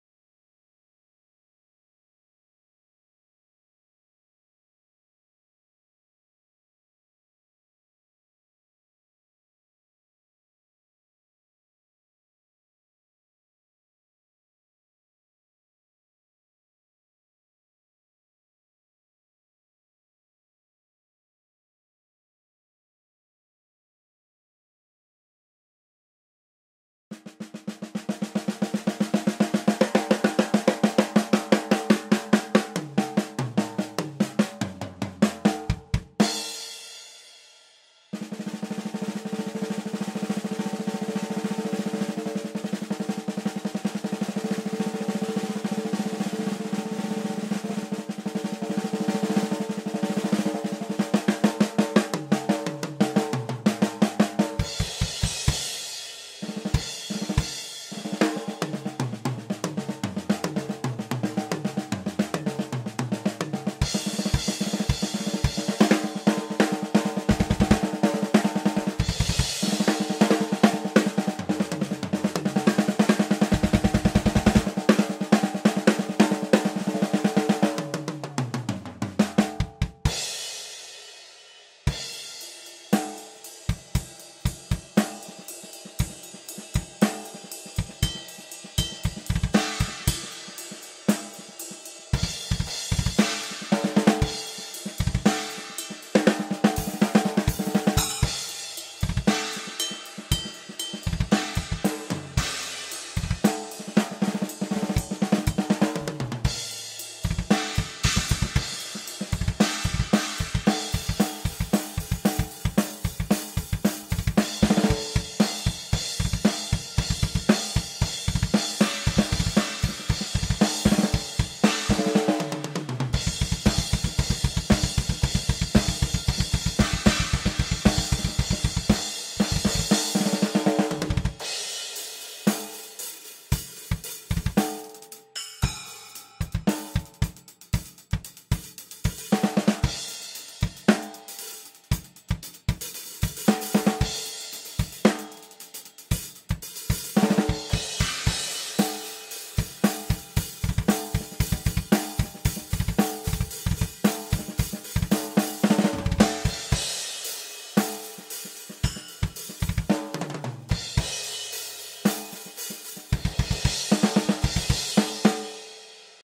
Pearl Steel Shell S-6114D 14x6.5 Japan
Запись произведена на 4 микрофона без пост-обработки
snare drum CAD TSM 411
bass drum AKG D11
overheads Behringer C-2